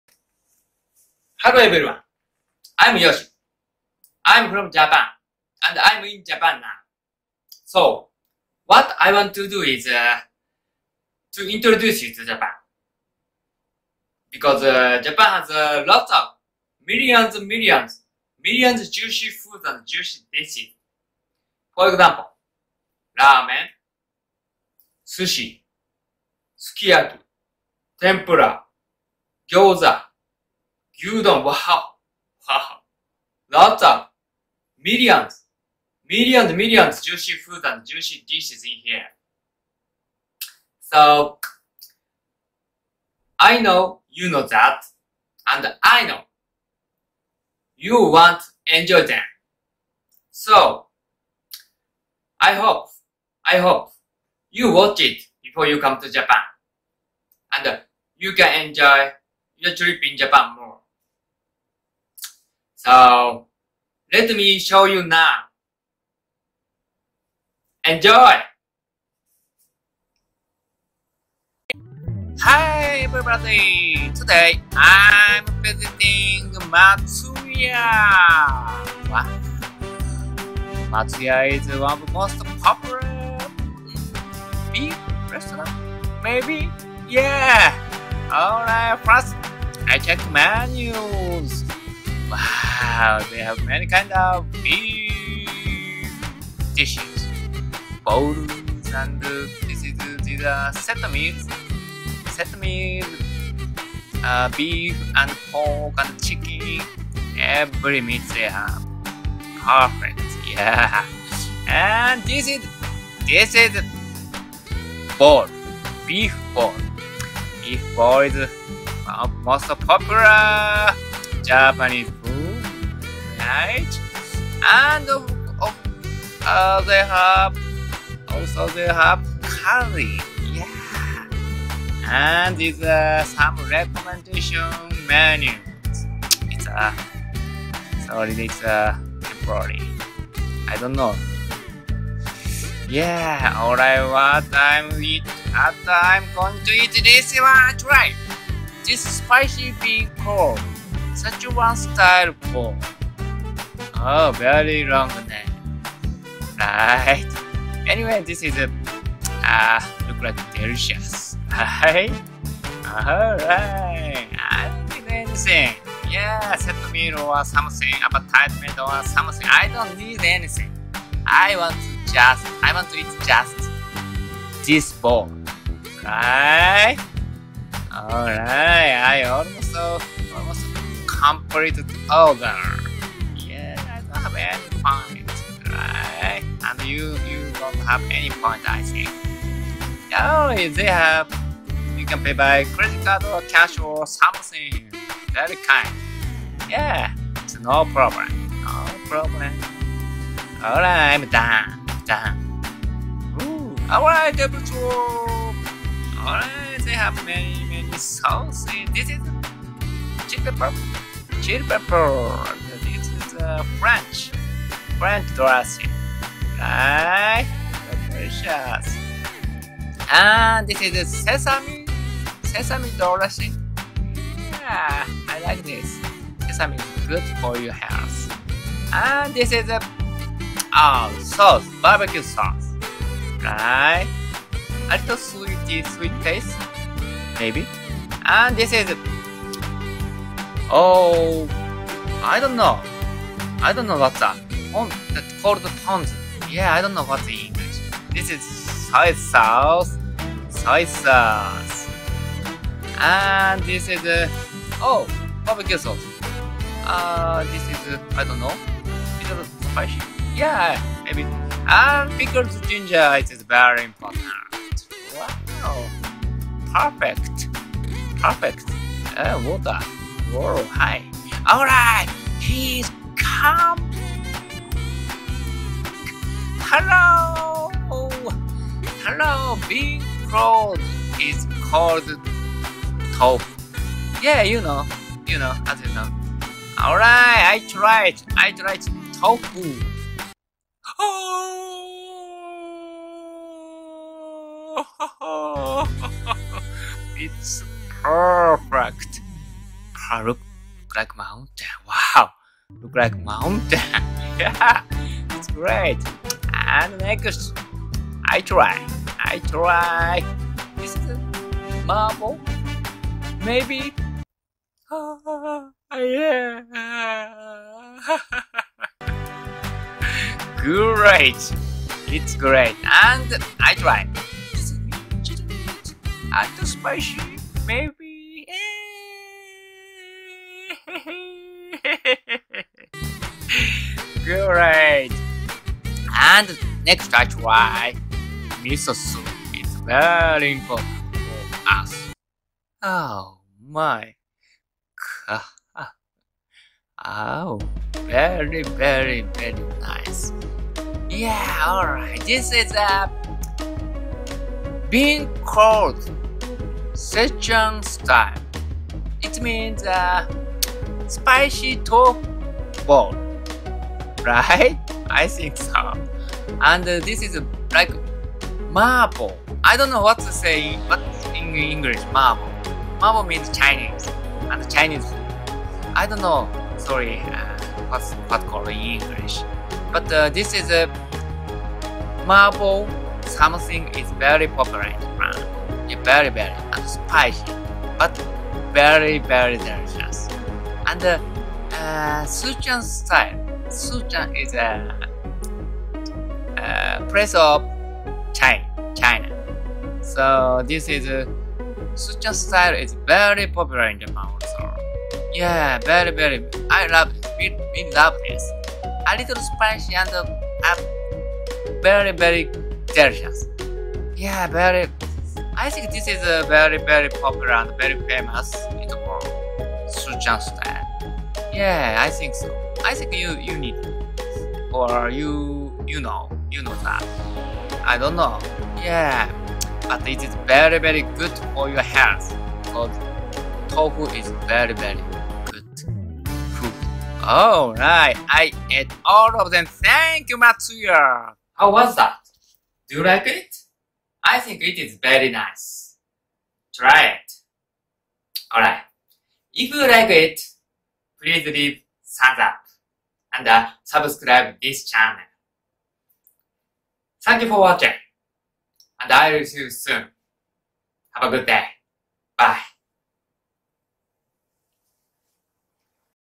#115 Spicy Bean Curd Szechuan-style Bowl in Matsuya
I tried Spicy Bean Curd Szechuan-style Bowl in Matsuya at this time. Matsuya is one of the most popular restaurants in Japan. They have a lot of branches all over Japan. So I think you can find them easily wherever you are in Japan. Enjoy!